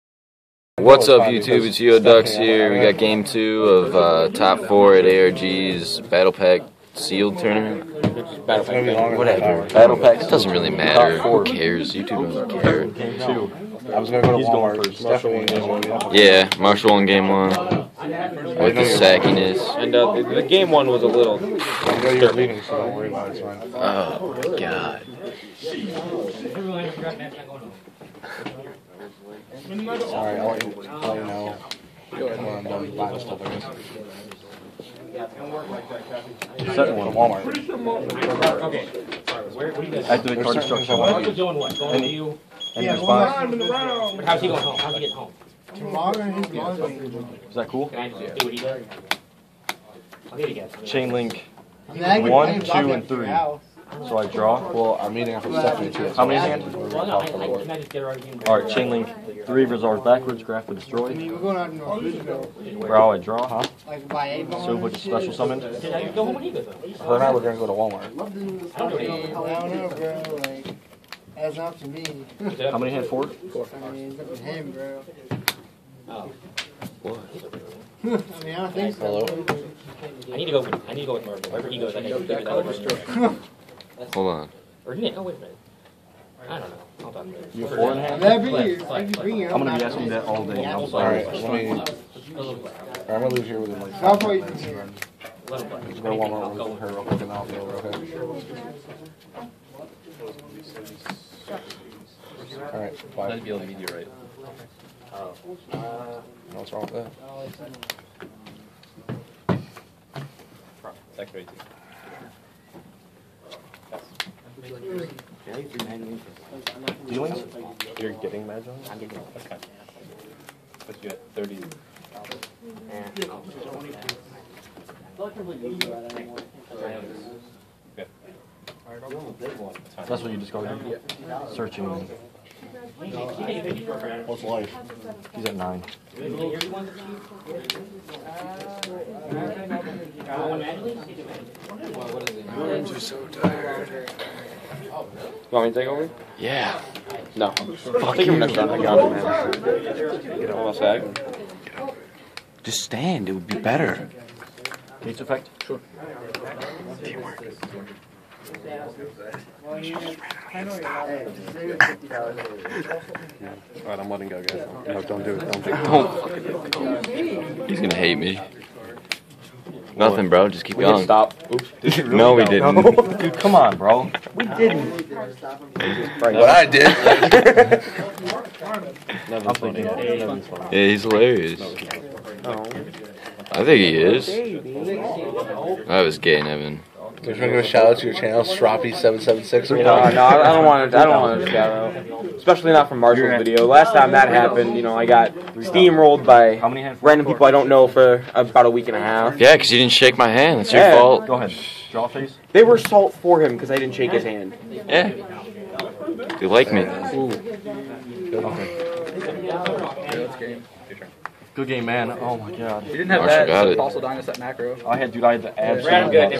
Top 4 Dark World vs. Macro Rabbit Game 2 ARG Sealed Tourny March 2013
Hey YouTube here with game 2 of the top 4 match of the ARG Sealed Tourny! Dark World (me) vs. Macro Rabbit. If you liked the video please be sure to give it a "LIKE"! -DarkWorldMOFO